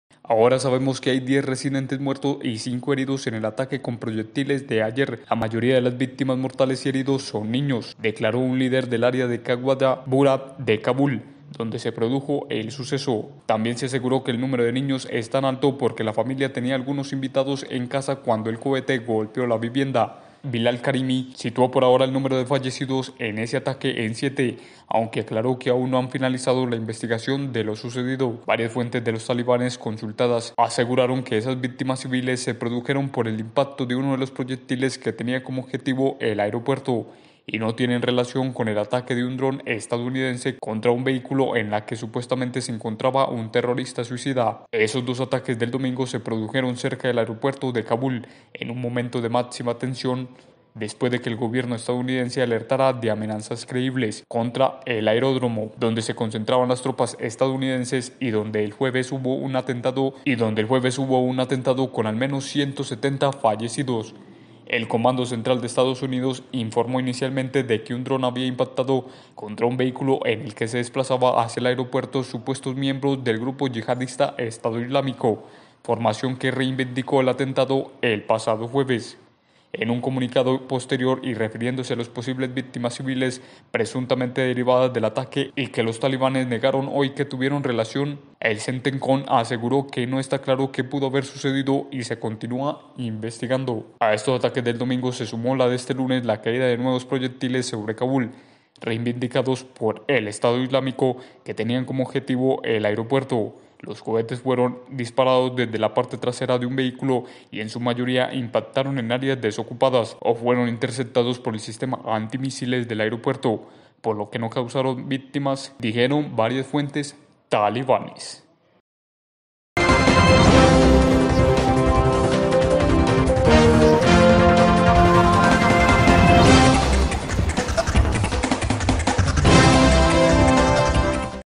AUMENTAN A 10 LOS MUERTOS POR EL ATAQUE CON PROYECTILES EL DOMINGO EN KABUL
El número de víctimas por el impacto el pasado domingo de un proyectil en una vivienda en Kabul aumentó a diez muertos y cinco heridos, en su mayoría niños, según el último balance disponible este lunes por fuentes próximas a la tragedia.